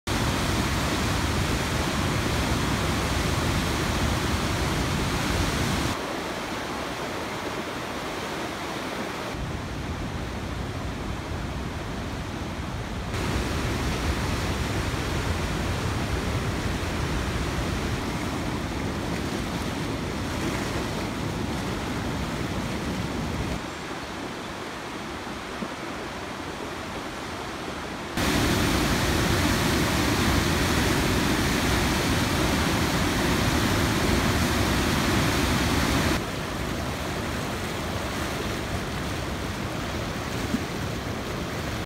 Hochwasser - Bregenzer Ache in Egg Junkerau
Auch die Bregenzer Ache in Egg Junkerau kämpft mit dem Dauerregen.